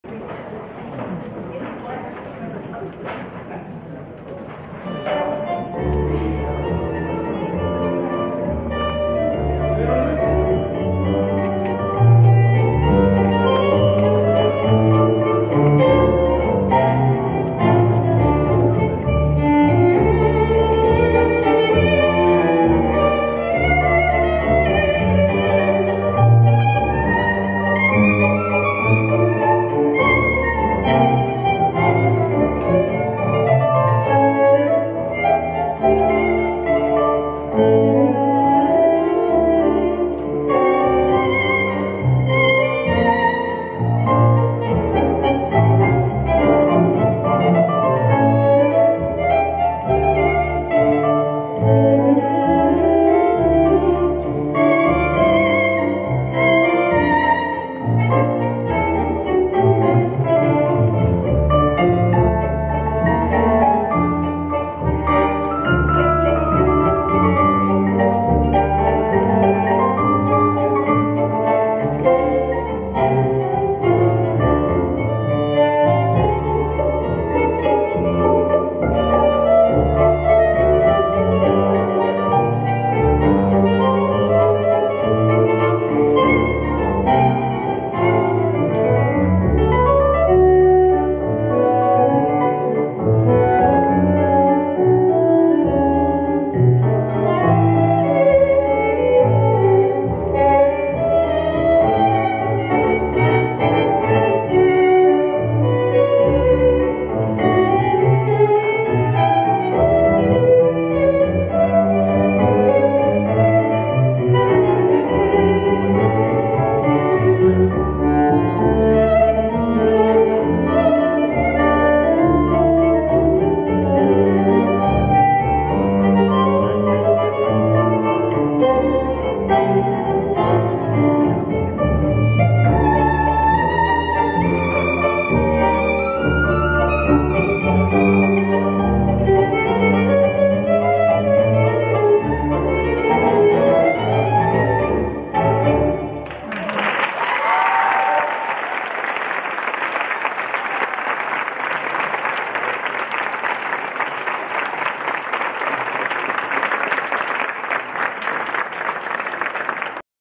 Jairelbhi y George Furlong Tango Performance as part of the "Celebrating Argentina - An Evening of Tango, Literature, and Food" presented by The Argentine Studies Program of the LLILAS Benson Latin American Studies and Collections at the University of Texas at Austin on Thursday, October 16, 2014.